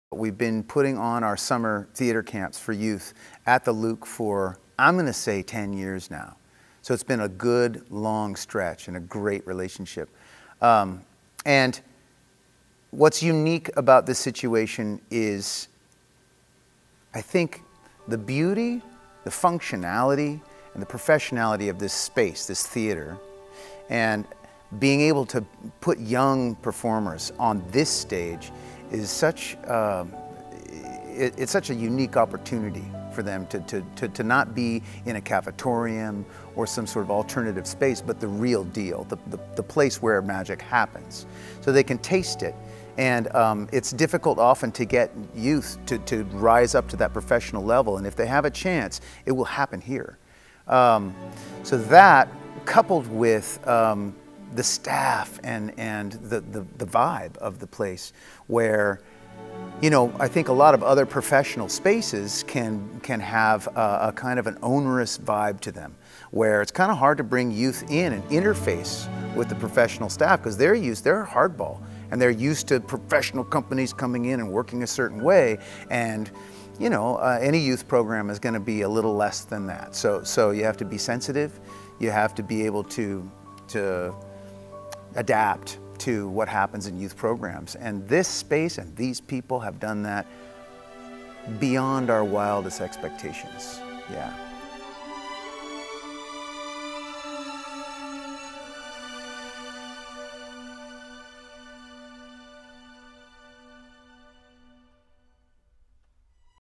Michael Andrews Boxtales
Michael Andrews speaks on The Luke theatre's impact on Santa Barbara's youth theatre and how the Boxtales Theatre Company have found a summer home in The Luke.